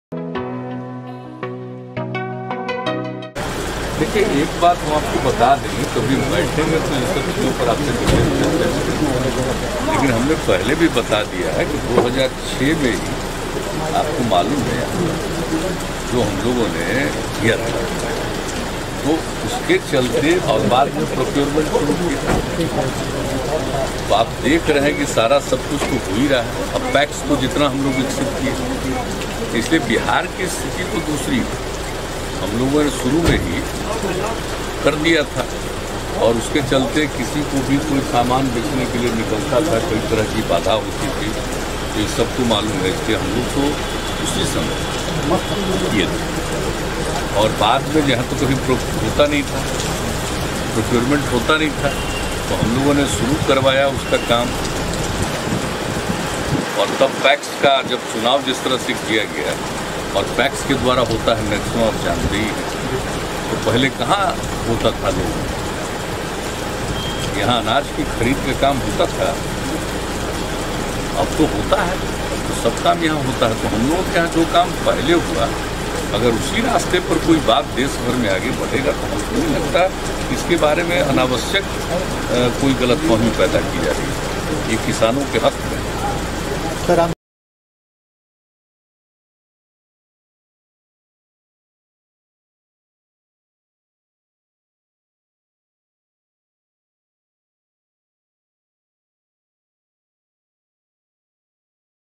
CM Nitish Kumar ने किया किसान बिल का समर्थन, बोले- किसानों के हित में है बिल
बिहार के मुख्यमंत्री नीतीश कुमार ने कृषि बिल को किसानों के हक में बताया है... सीएम नीतीश ने कहा कि हमने वर्ष 2006 में ही पैक्स के द्वारा प्रोक्योरमेंट शुरू किया। पैक्स को हम लोगों ने विकसित किया इसलिए बिहार की स्थिति दूसरी है। सीएम ने कहा कि कृषि बिल के बारे में अनावश्यक गलतफहमी पैदा की जा रही है। ये बिल किसानों के हक में है.